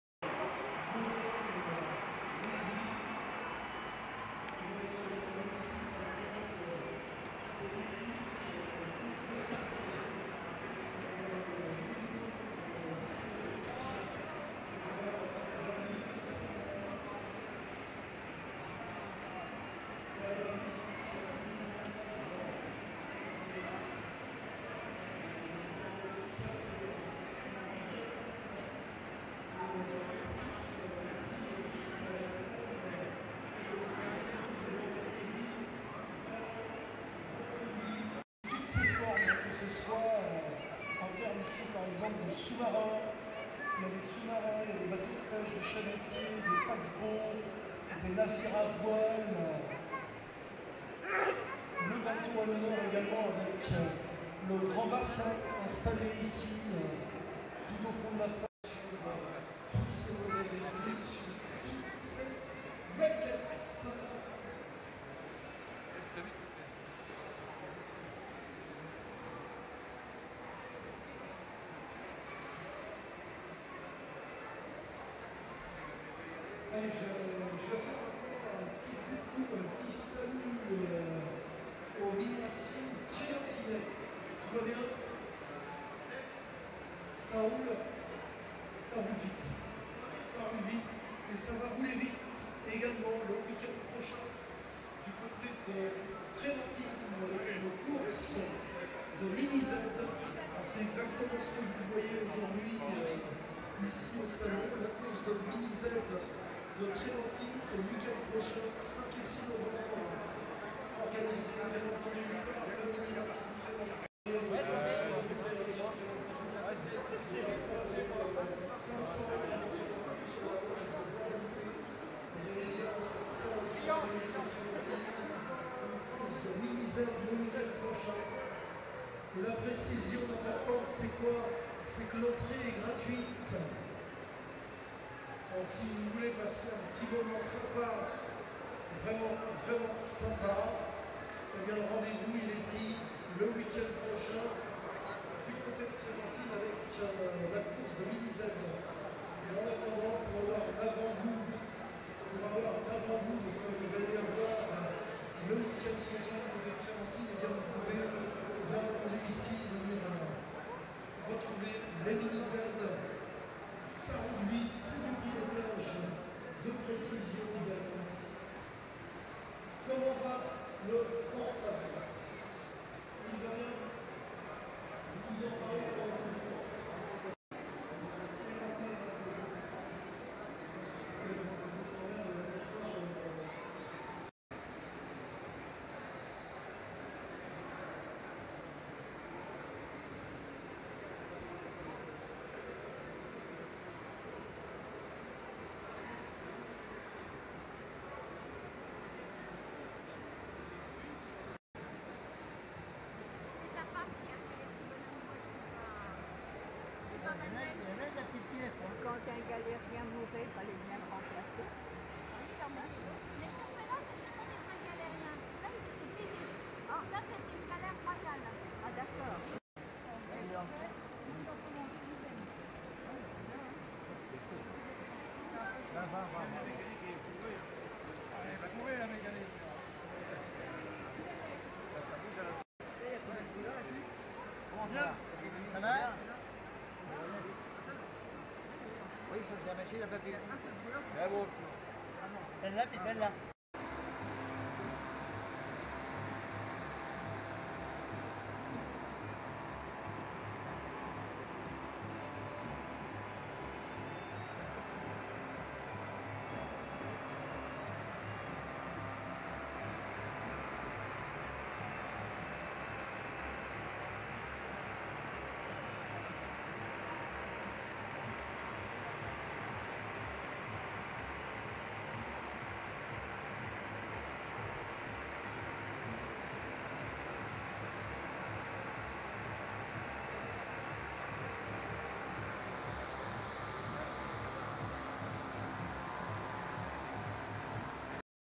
Cholet 2011 , salon du jeu et du modelisme vu par le Breizh RCB
quelques stands pris avant la venue du nombreux public .